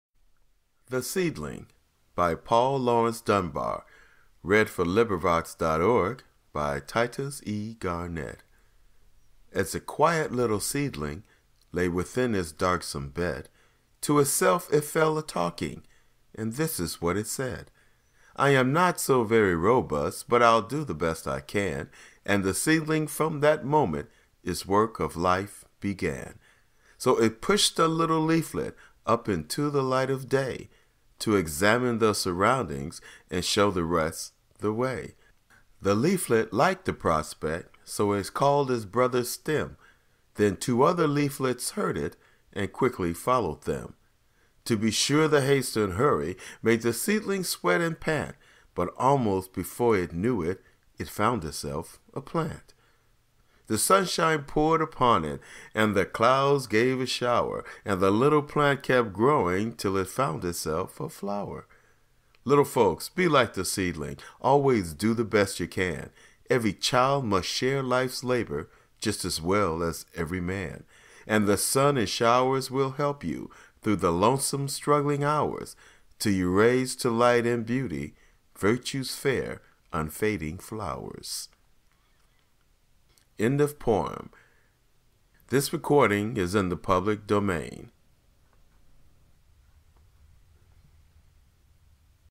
"The Seedling" by Paul Laurence Dunbar
Title: The Seedling
Author: Paul Laurence Dunbar
Audio Source: Librivox.org
Image Source: Pixabay.com
"Elementary Poetry Volume 4 – Advancing in Poetry" by Sonja Glumich available